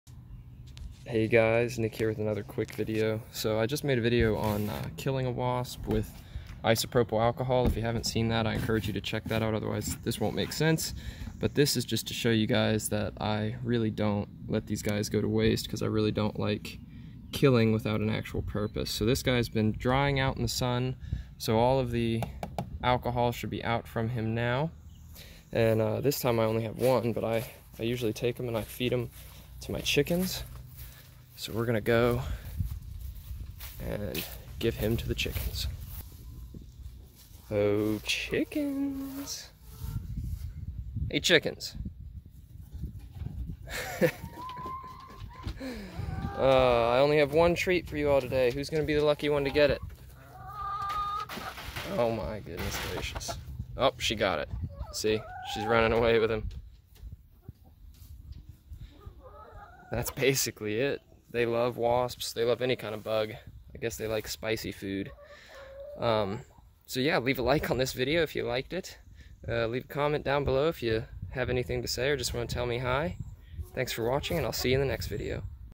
Feeding a Wasp to My Chickens
Chickens love bugs and that extends to wasps. Watch my chickens go crazy over a wasp that I killed in an earlier video.